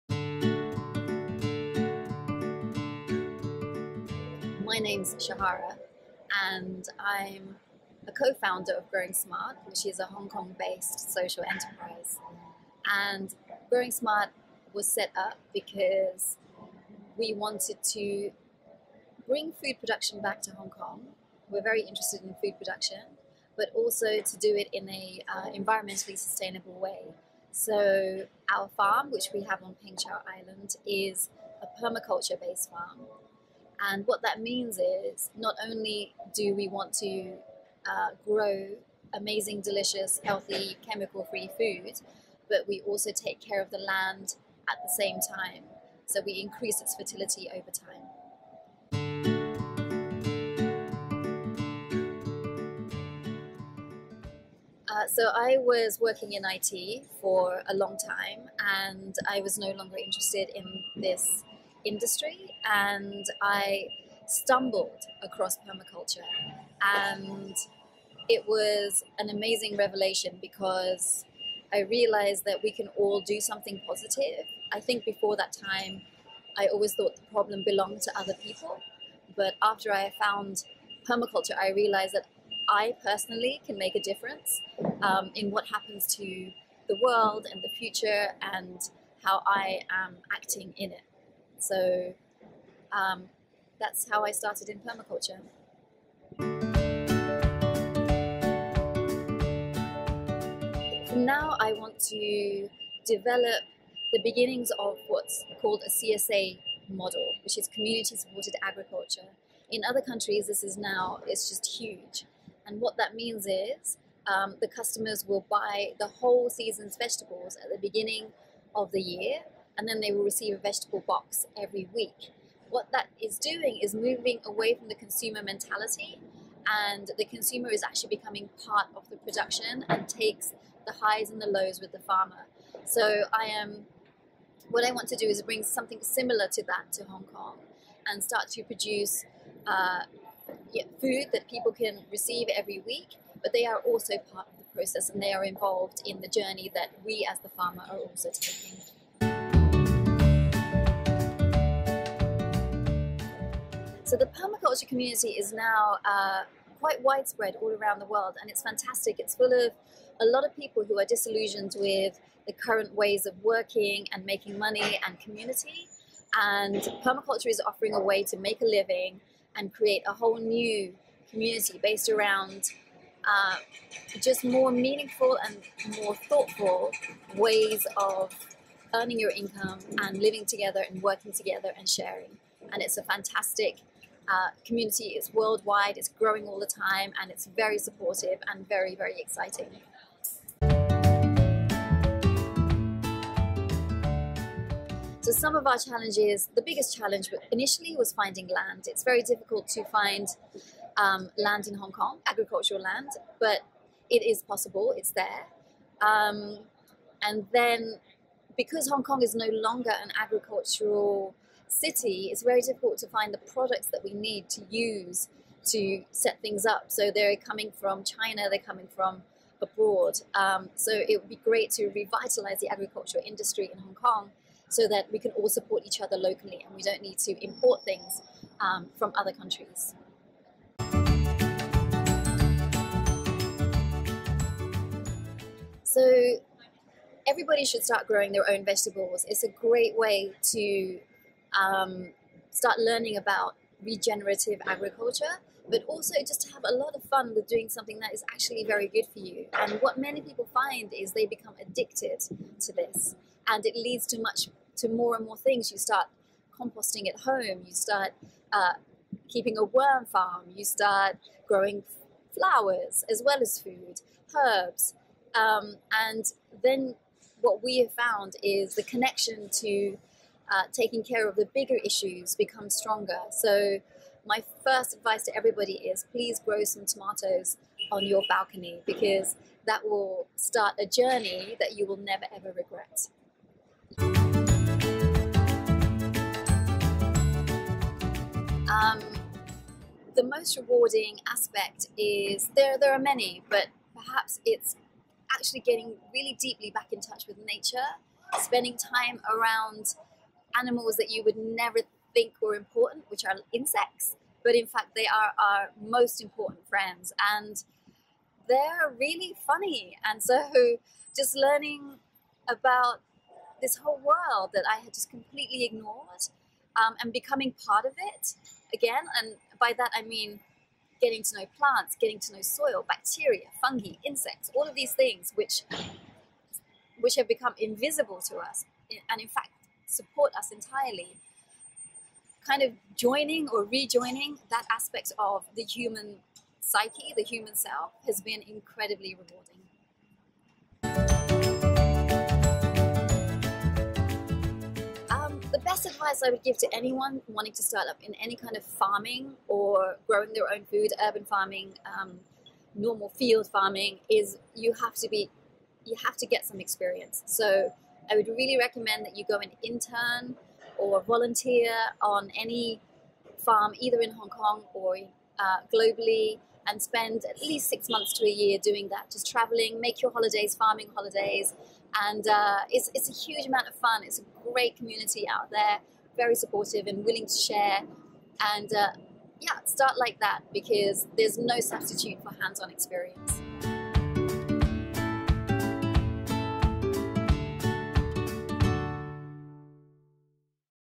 Interview with Shahara Khaleque, GrowingSmart
Meet Shahara Khaleque, Co-founder of GrowingSmart, whose permaculture-based farm on Peng Chau Island is not only focused on producing delicious, healthy, chemical-free food, but is also re-engaging consumers in the farming process. FOLLOW HER PROGRESS ON SPARKRAISE